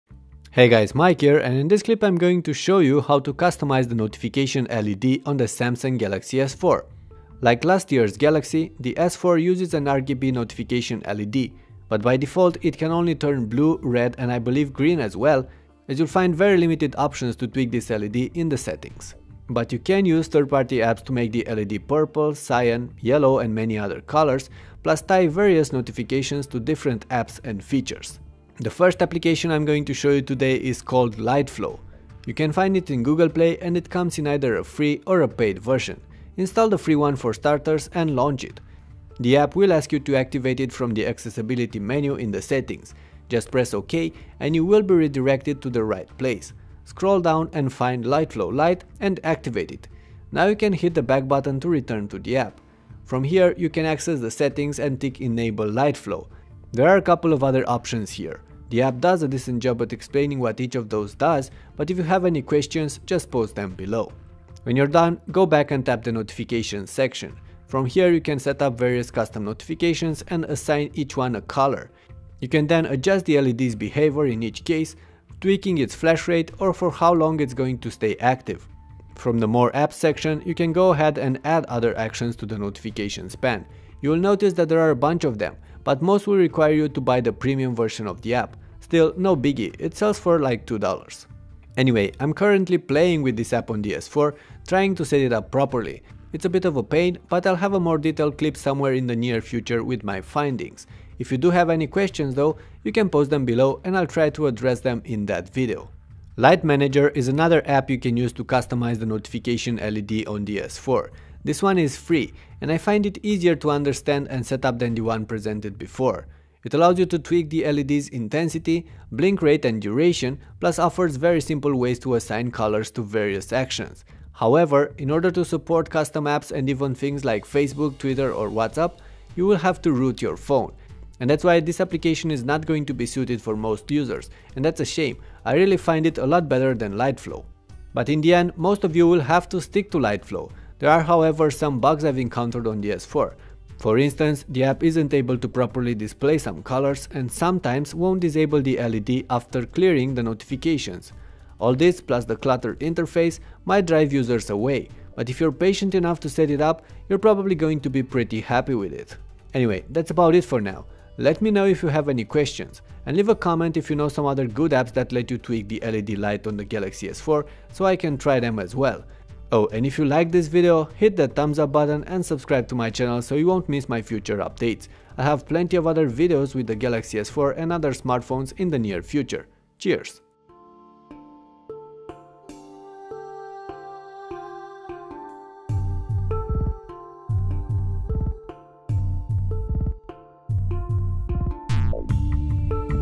Samsung Galaxy S4: how to tweak the notification LED
The Samsung Galaxy S4 comes with a LED notification light which can be tweaked to extreme by using third party apps. Here's our Samsung Galaxy S4: how to tweak the notification LED video. Let us know if you know any other means of tweaking the LED on your SGS 4.